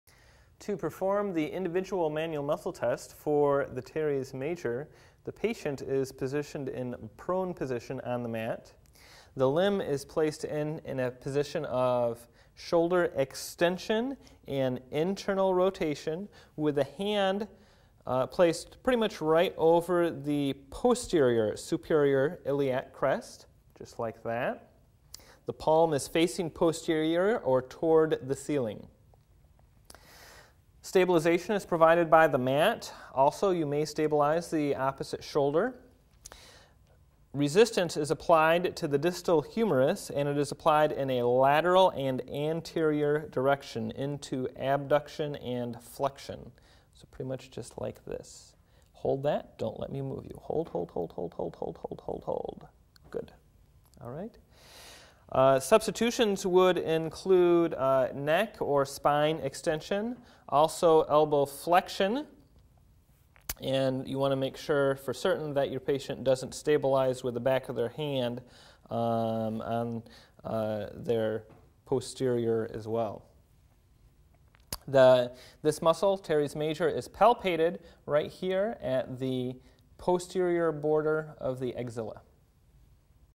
MMT Individual Muscle Teres Major
How to evaluate teres major strength with Manual Muscle Testing (MMT).